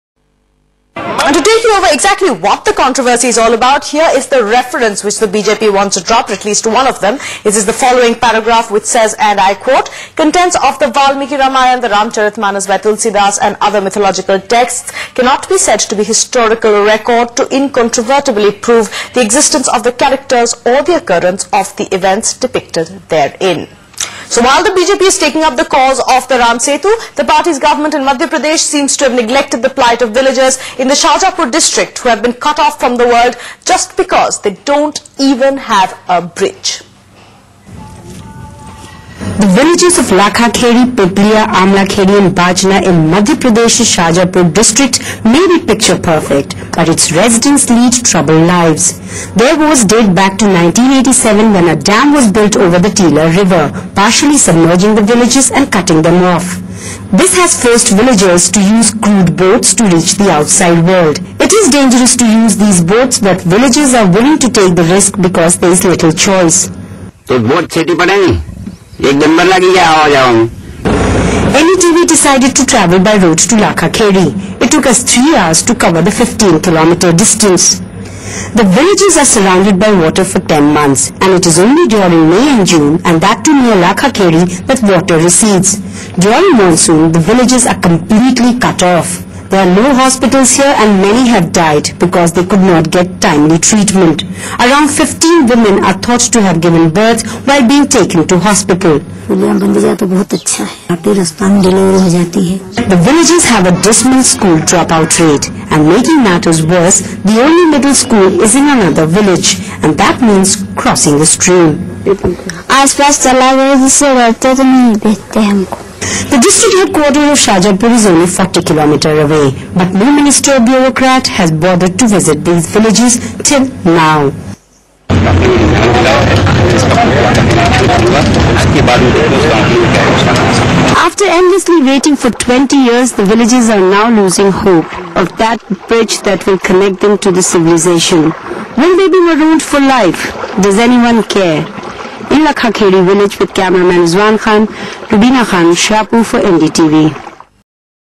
Shajapur villagers forced into isolation
Even while the BJP is taking up the cause of the Ram Setu, the party's government in Madhya Pradesh seems to have neglected the plight of villagers in Madhya Pradesh's Shajapur district.
The villages of Lakha Khedi, Pipliya, Amla Khedhi and Bajna may be picture perfect but its residents lead troubled lives.